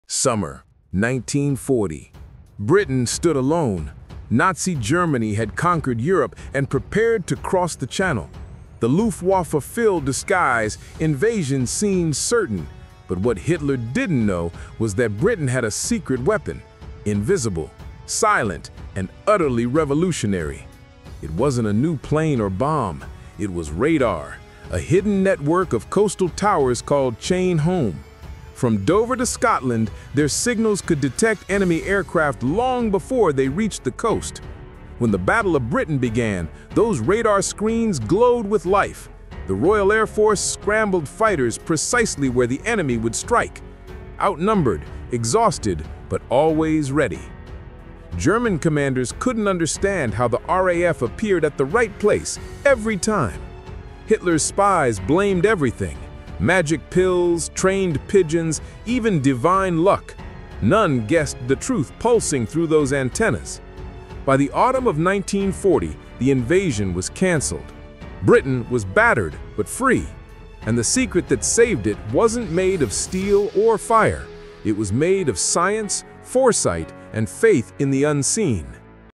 The Secret That Saved Britain from Invasion
In 1940, Britain faced the Luftwaffe alone. But an invisible weapon — radar — turned the tide of war. Discover how science and courage saved a nation from invasion.